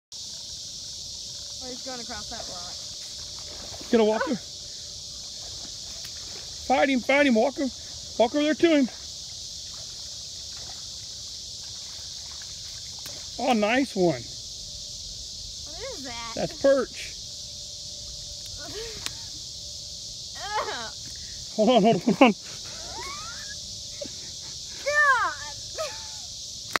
Fishing girl
Gabby brining in a big carp on the Wabash River